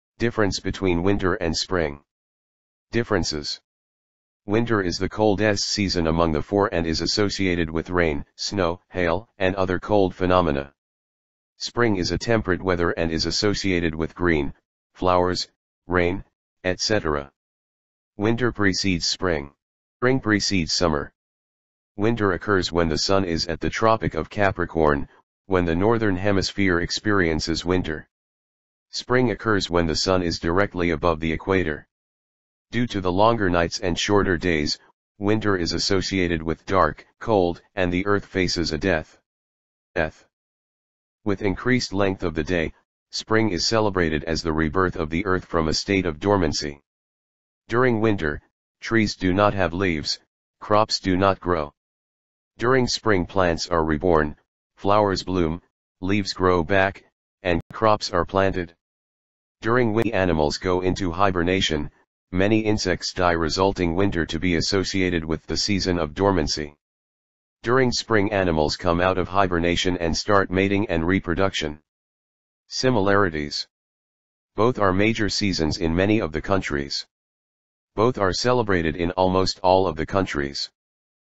Differences of Winter and Spring
Winter, on the other hand, occurs when the sun is at the tropic of Capricorn and the North Temperate Zone experiences winter. Spring means more daylight while winter means less daytime and more night time. Spring is, in fact, the season in which vegetation begins to appear.The differences between spring and autumn include temperature, time of year and length of day. Spring is the season that happens after winter, while autumn is the season that happens after summer. ... The first day of autumn is September 22 in the Northern Hemisphere and March 20 in the Southern Hemisphere.The passing of a year can bring a marked change in the weather and the surrounding environment. The four seasons — winter, spring, summer, autumn — can vary significantly in characteristics, and can prompt changes in the world around them. Let's take an overview of these four separate periods.As nouns the difference between monsoon and spring. is that monsoon is any of a number of winds associated with regions where most rain falls during a particular season while spring is a leap; a bound; a jump.Although spring has arrived, it may not feel that way for many in the United States and Canada who have had to put up with unusually cold temperatures. ... This flow of air, known as the jet stream, usually brings more warm air from the South as the Northern Hemisphere begins to heat up in the spring.